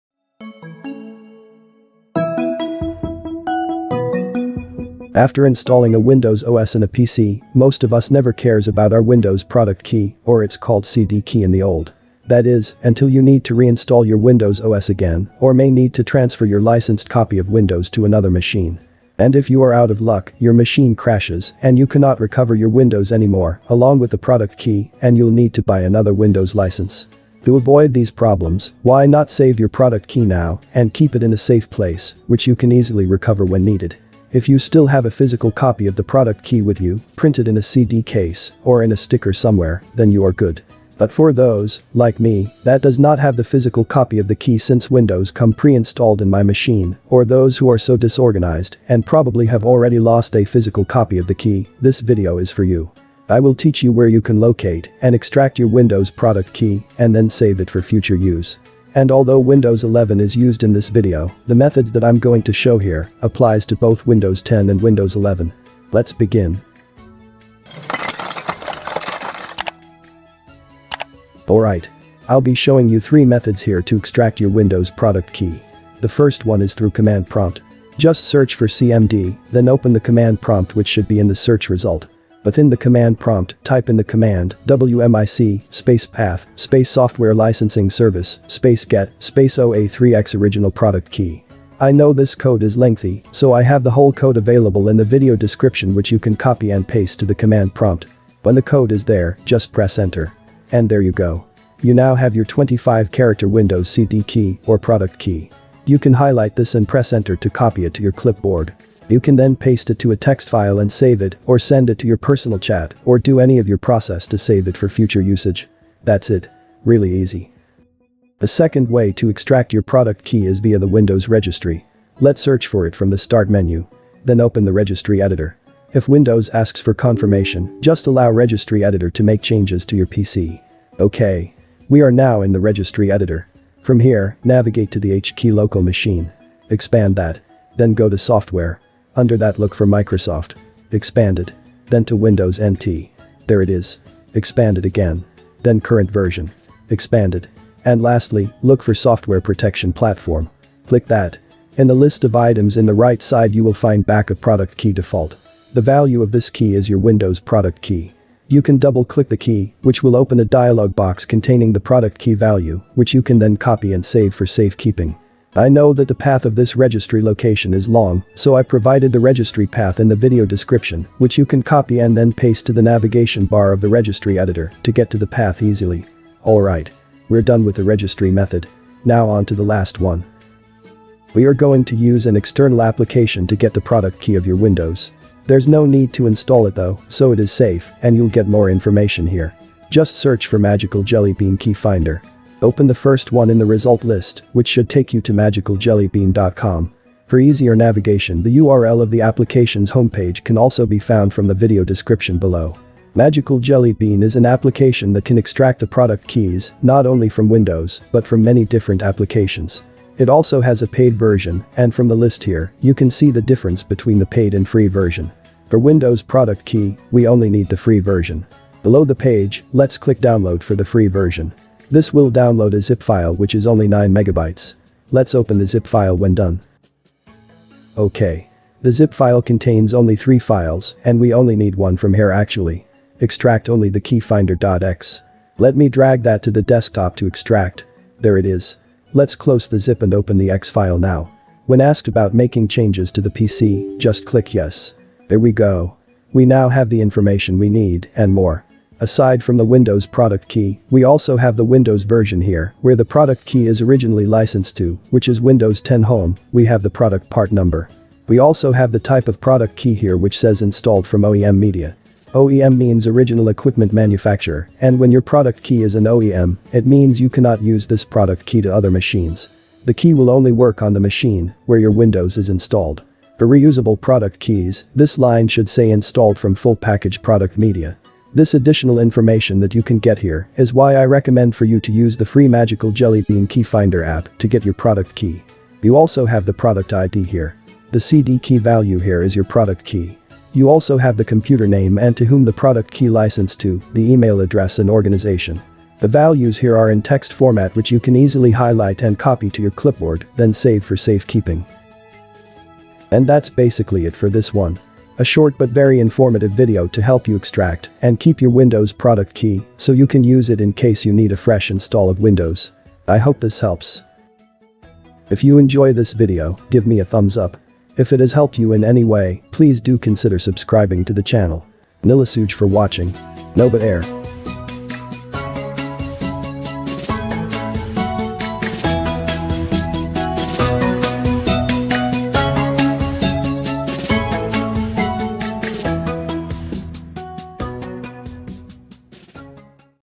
Locate The Product Key Of Your Windows 10/11 OS
Registry Location Path:
   HKEY_LOCAL_MACHINE\SOFTWARE\Microsoft\Windows NT\CurrentVersion\SoftwareProtectionPlatform

00:00 Intro
01:11 Via Command Prompt
01:58 Via Registry
03:01 Via Free External App (recommended)
05:12 Outro